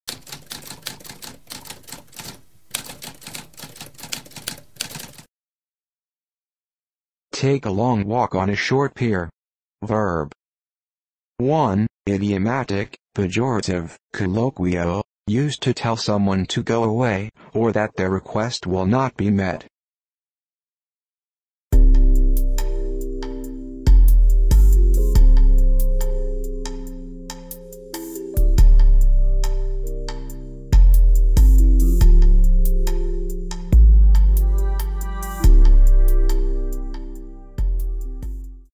What does take a long walk on a short pier mean?
A spoken definition of take a long walk on a short pier.

Intro Sound:
Typewriter - Tamskp